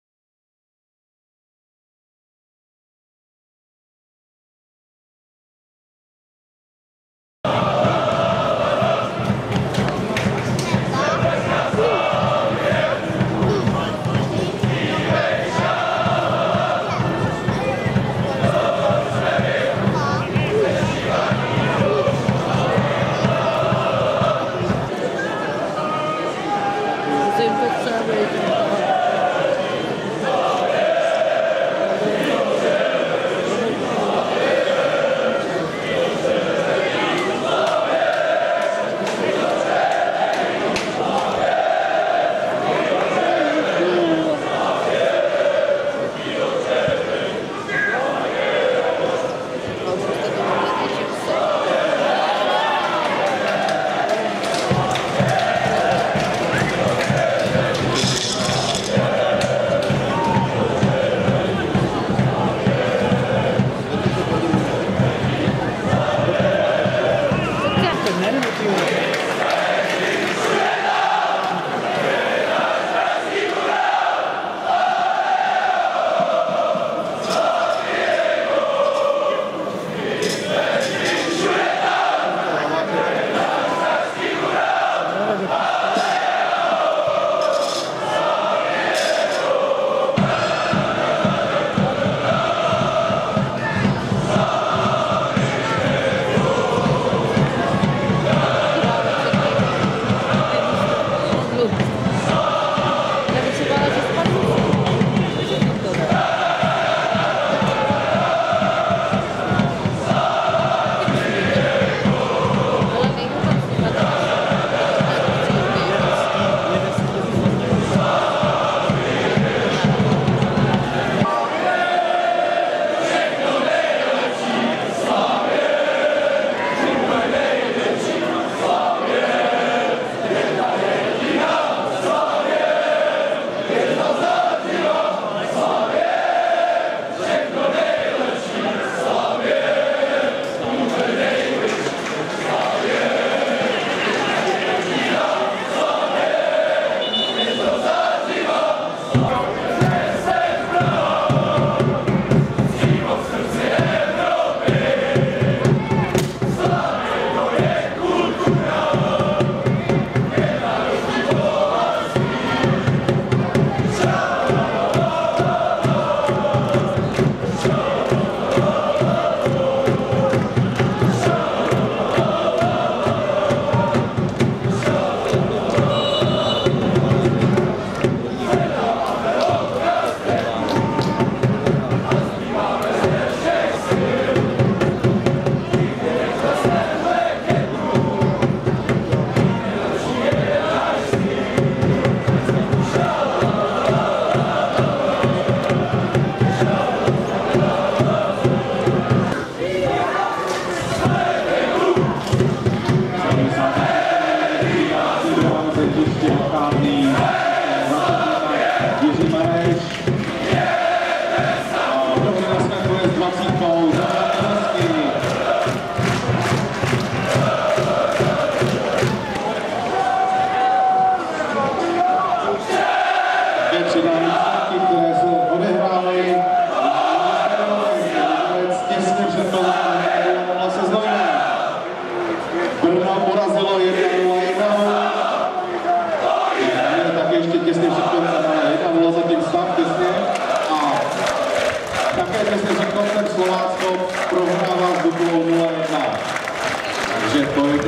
Příbram - SLAVIA PRAHA, 2.11 2013
Tribuna Sever na nedalekém výjezdu v Příbrami.